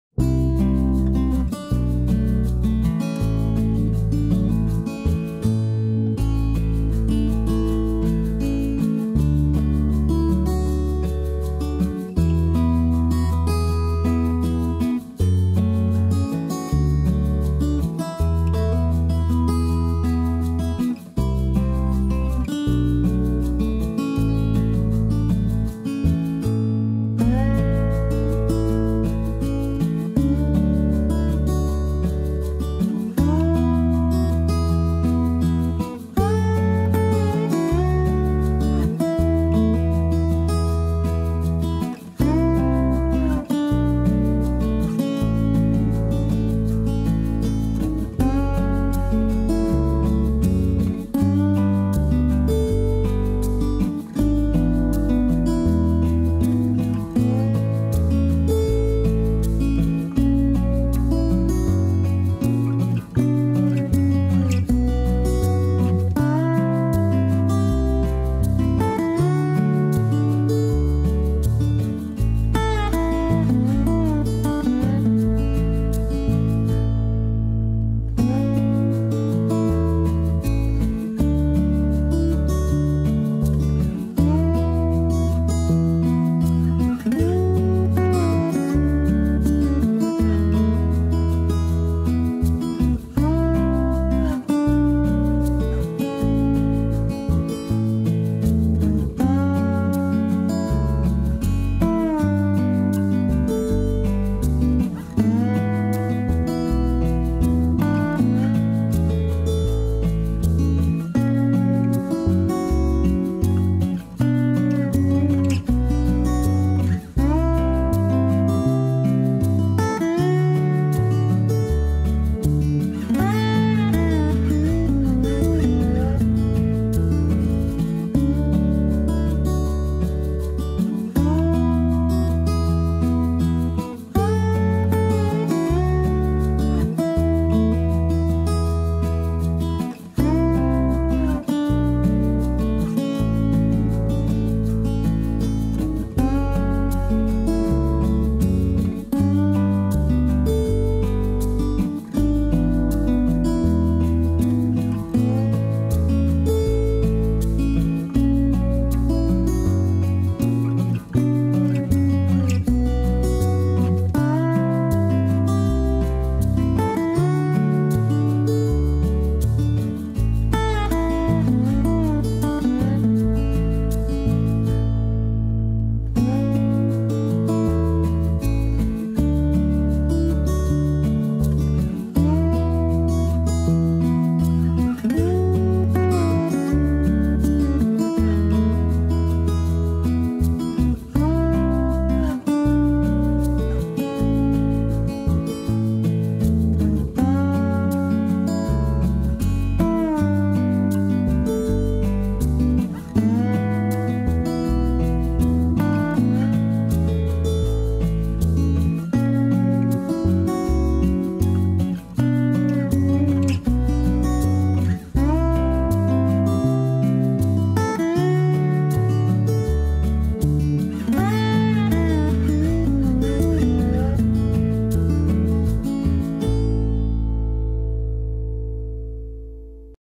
Sleeping cats
Patrick and Otis are sleeping. Cats are great lounge lovers. They enjoy every minute of slumber